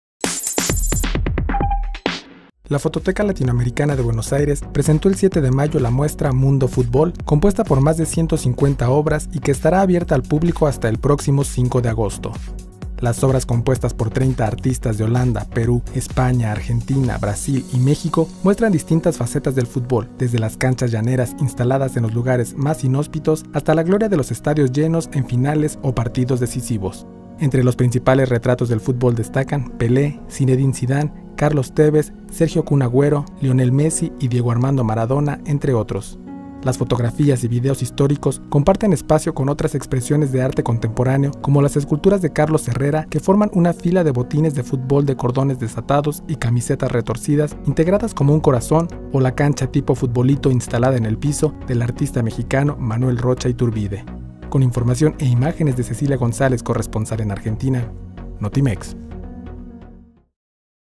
Exhibe Fototeca Latinoamericana "Mundo futbol" en Buenos Aires
La exhibición con obras de 30 artistas de diferentes nacionalidades, muestra distintas etapas del futbol, desde las canchas hasta los estados profesionales